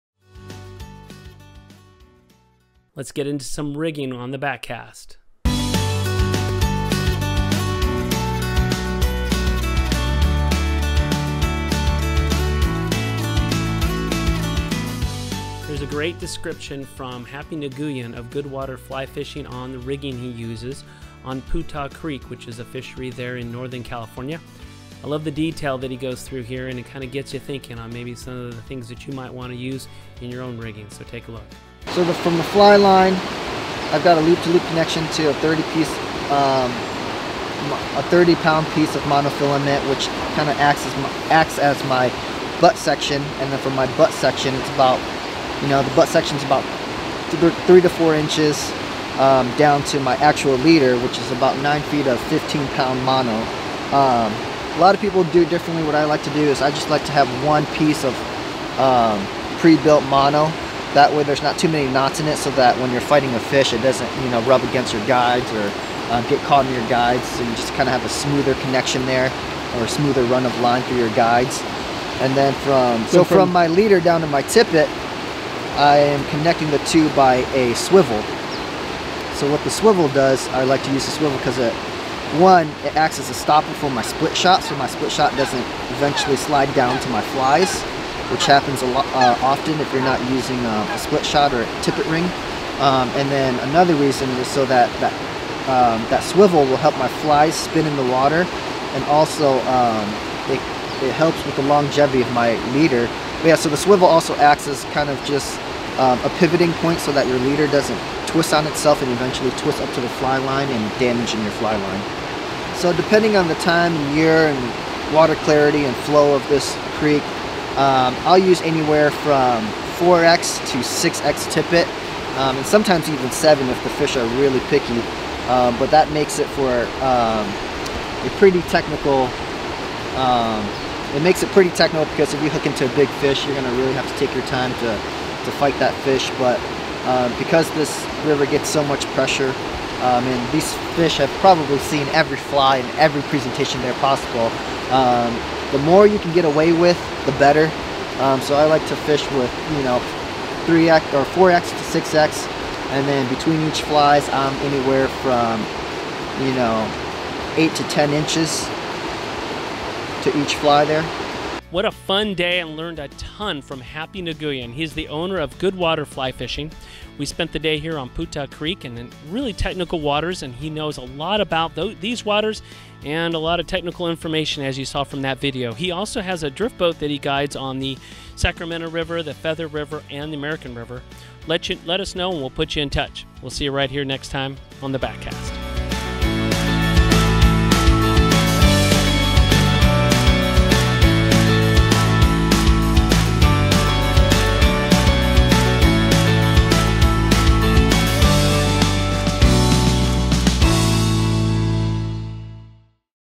Rigging for Putah Creek Fly Fishing
Here's a great description from fly fishing guide Happy Nguyen, the owner of Goodwater Fly Fishing on the rigging he uses on Putah Creek, which is a fishery there in Northern California. I love the detail that he goes through here, and it kind of gets you thinking on maybe some of the things that you might want to use in your own rigging. So take a look.